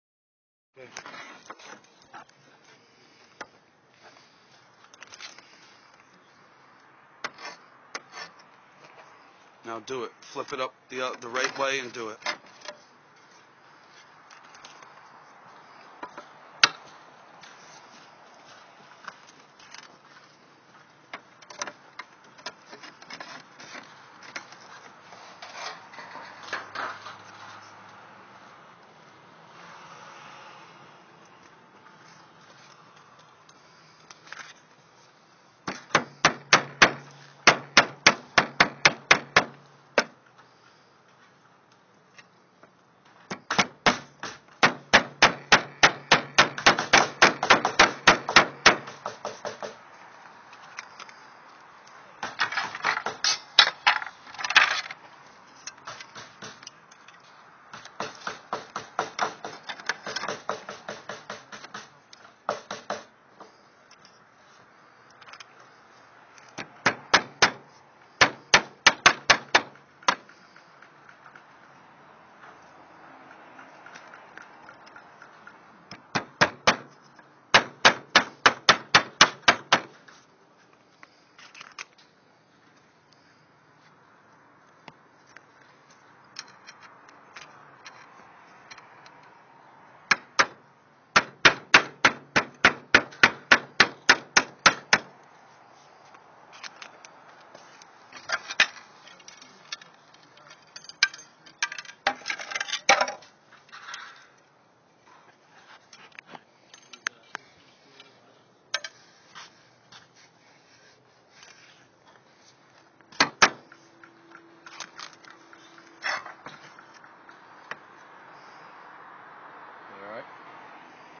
Slate repairs at the National Museum of Racing in Saratoga Springs - 7 of 9
Slate roof preservation in action video 7 of 9